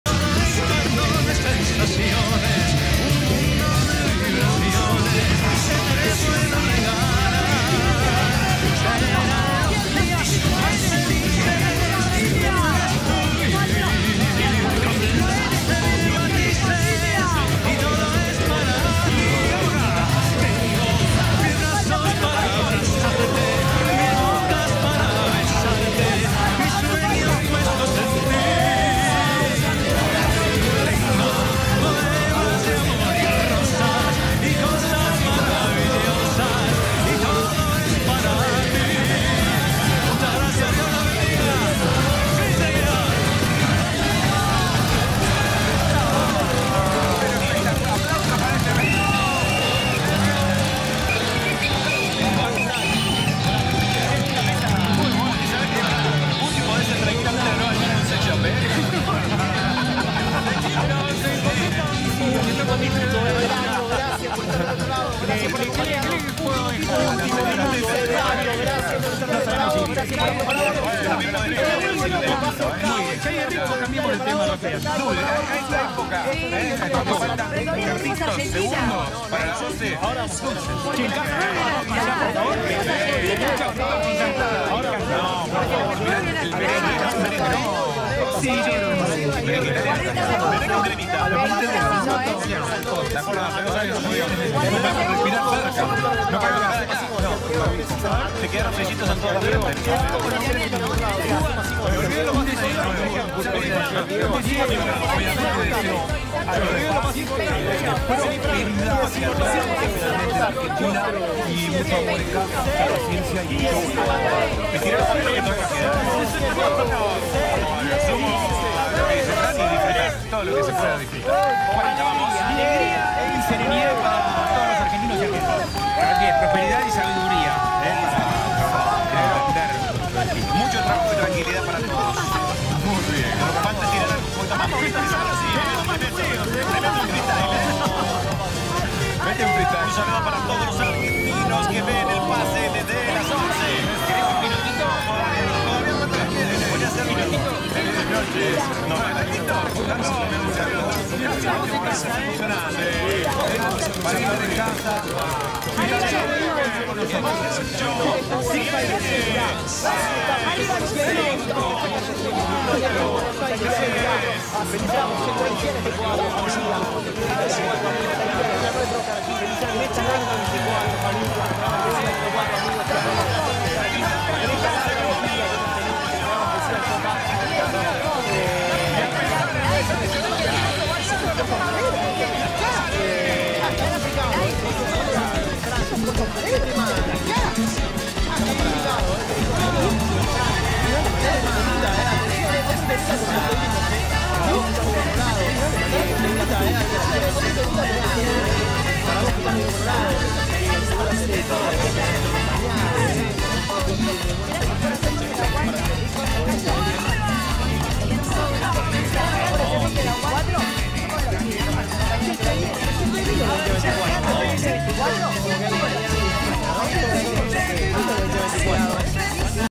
[Mosaico] CONTEO REGRESIVO PARA AÑO NUEVO EN LA TV ARGENTINA (31-12-2023/01-01-2024)
Las siguientes grabaciones fueron recepcionadas a traves de:

- C5N
Via Internet (Stream VLC)

- CRONICA TV HD
Via Internet (Live Streaming PhotocallTV)

- TN HD
Via Internet (Stream de YT)
Mencion Honorifica a: Mi amiga (Que empieza con E xd)
Por sugerirme hacer estas grabaciones